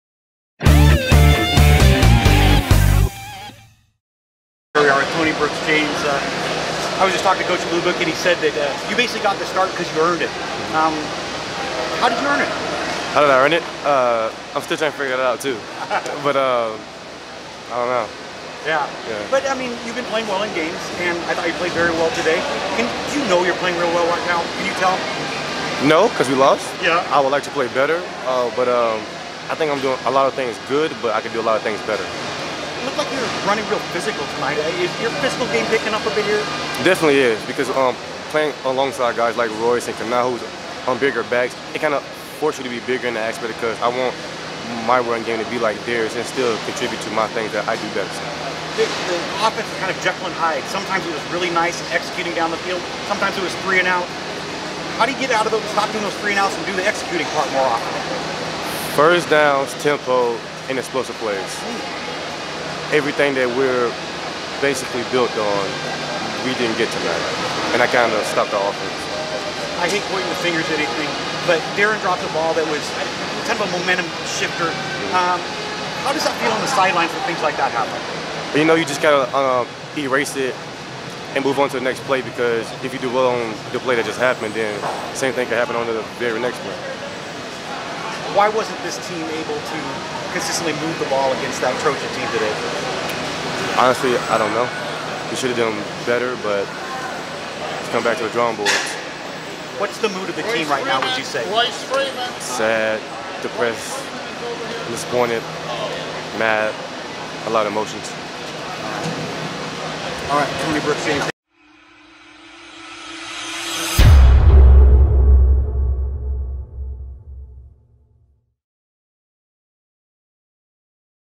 Tony Brooks-James post-USC
Talking with Oregon RB Tony Brooks-James following USC 2016.